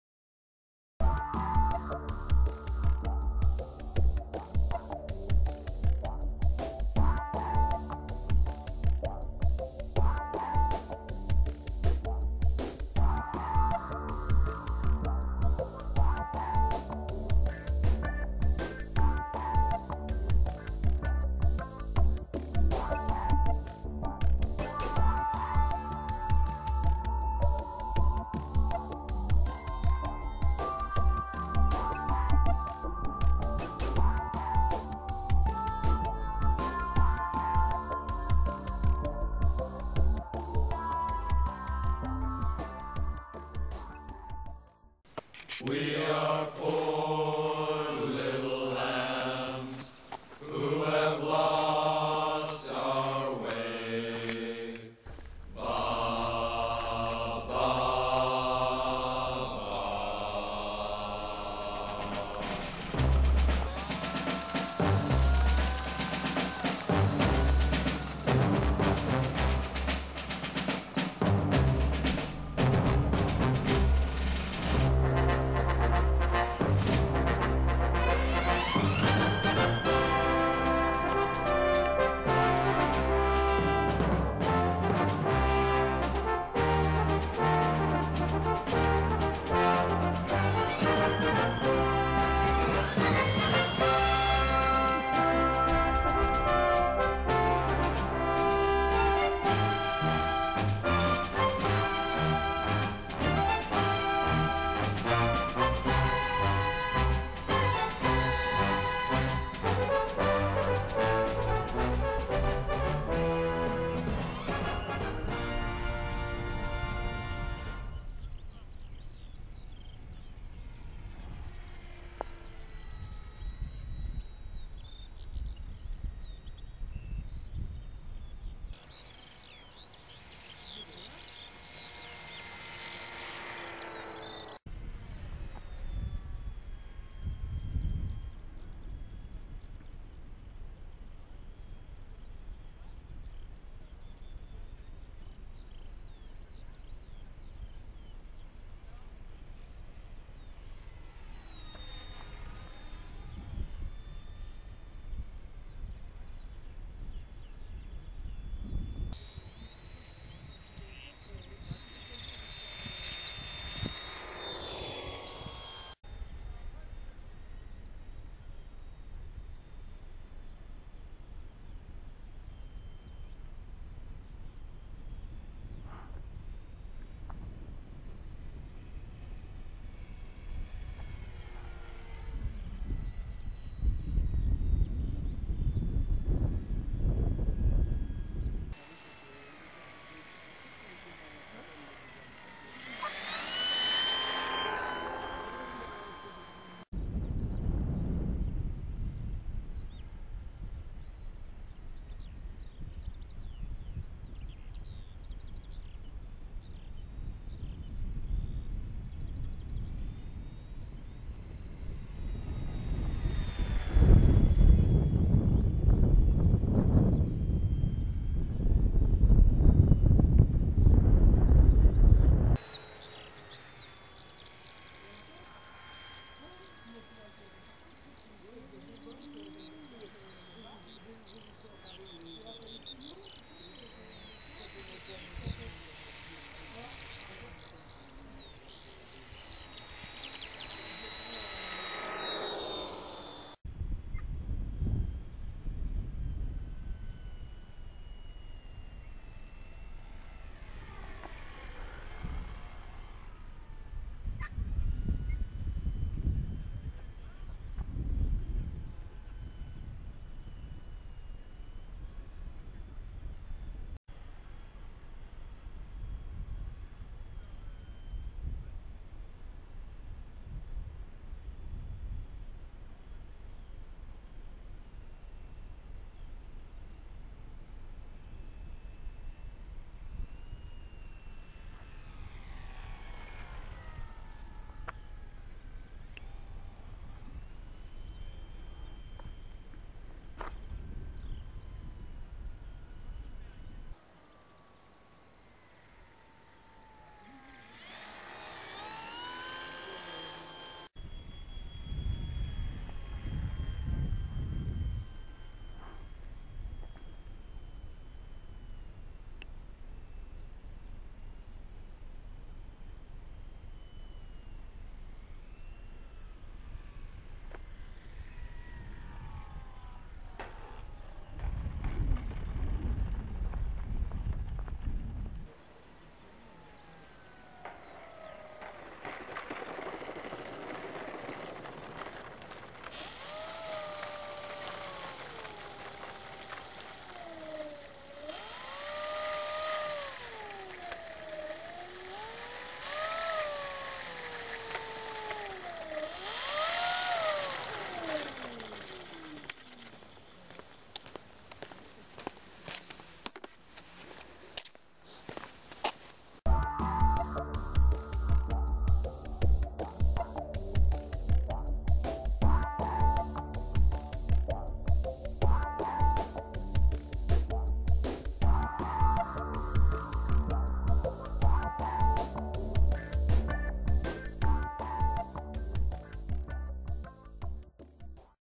Corsair Depron. Construction et premier vol.
Ayé!! Premier vol de mon Corsair en Depron tout juste terminé...
1.10m, 1Kg, train rentrant, Moteur 2836/750kv, hélice quadripale 10*8 le tout alimenté par une 3S 2200mAh.

j'expérimente aussi l'entoilage au papier kraft, et le rendu est plutot sympa...

Gros moment de stress après 6 mois de bricolage, vais-je le crasher au premier décollage?

Bin non... ça vole... Malgré un décollage pas terrible (va falloir que je m'habitue...)

Le comportement en l'air est agréable. mais ça allonge à l'atérro... Reste 2 ou 3 bricoles à terminer dont le cockpit, et revoir un peu le centrage...